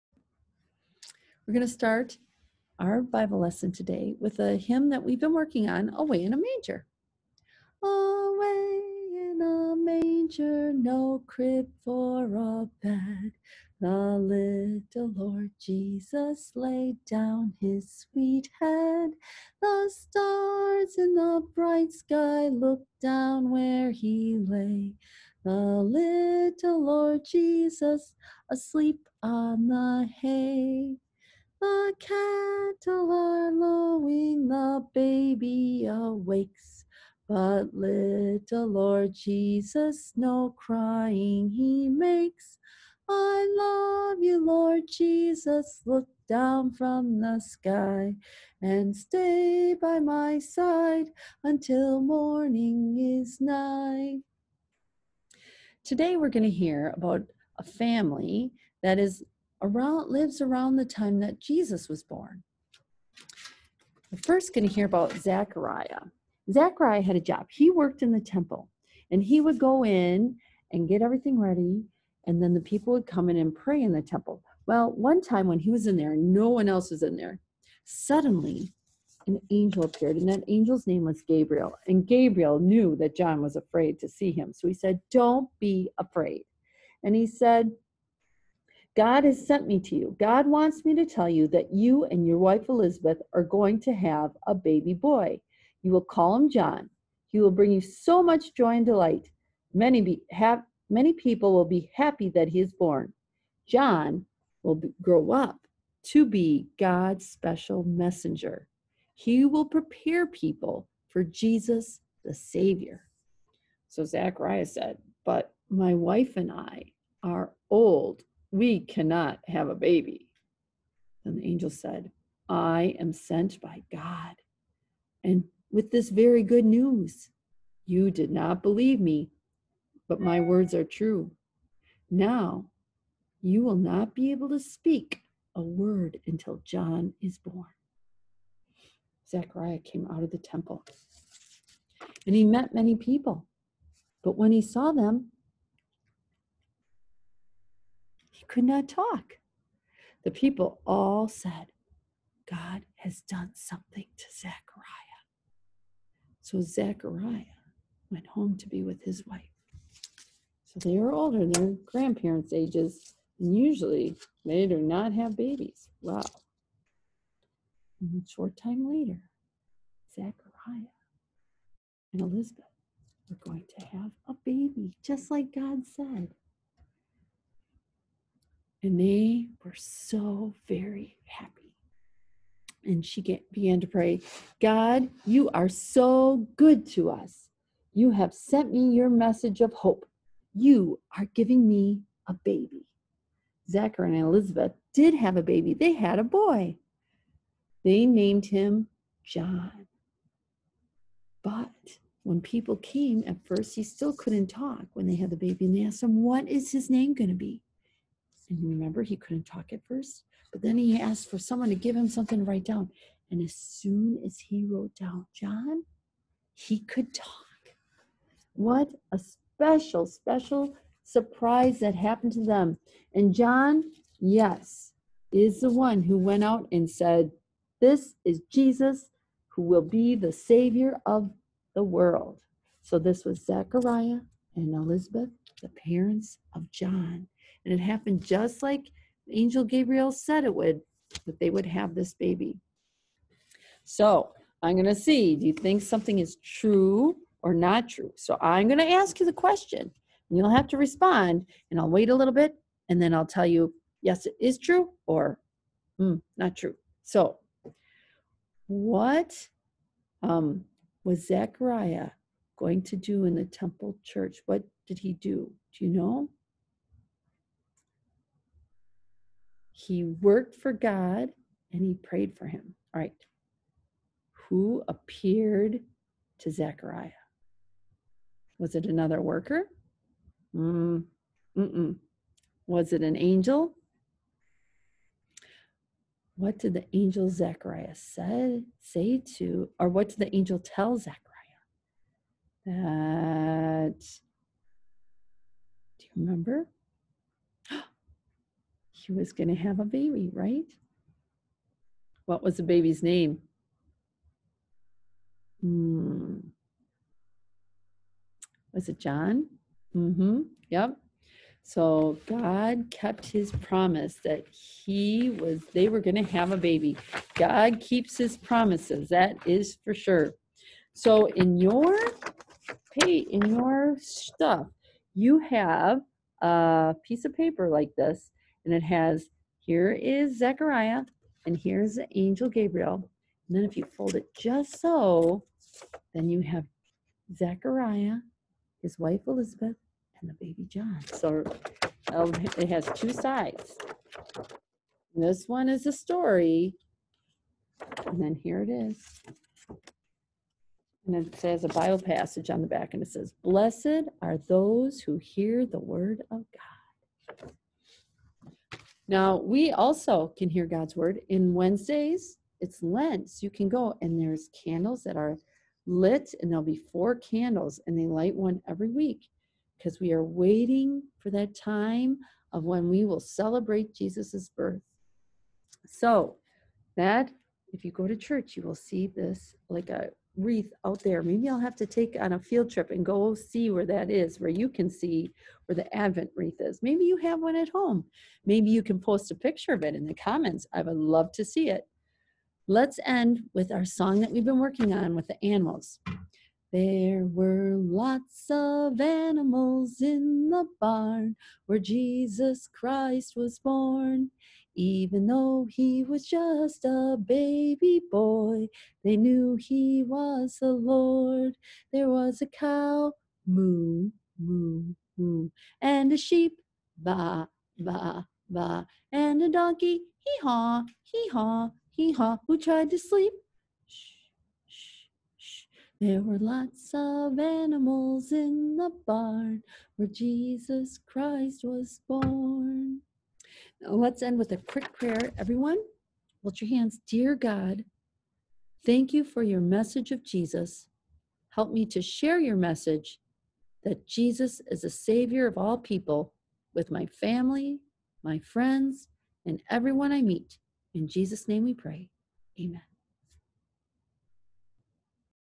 God Gets Ready to Send the Savior
Zechariah and Elizabeth and the birth of John. They couldn't wait for their son to be born.  It is the time of Advent at church.  We are waiting and remembering that the promise of a Savior was kept. We celebrate Jesus' birth at Christmas!